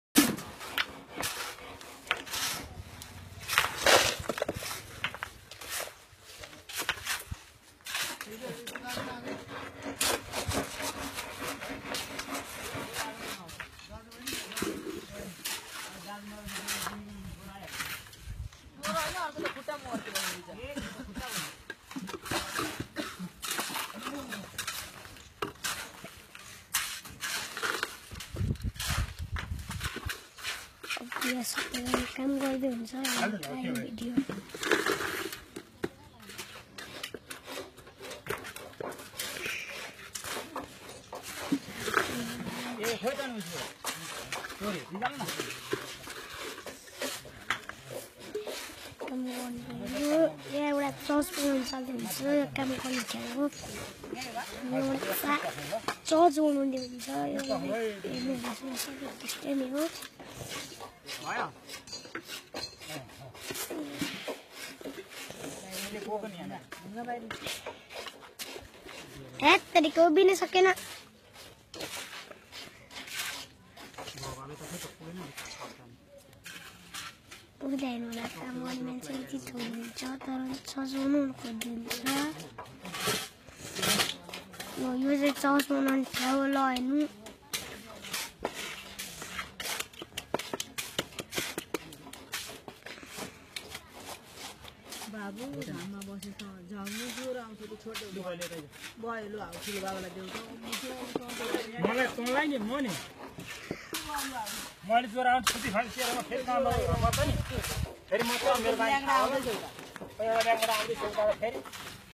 Dharan Ray of Hope church construction
Dharan Ray of Hope church construction is going on. Brother Youbraj Sherestha is a pastor of the church here. Please pray for him and this church construction.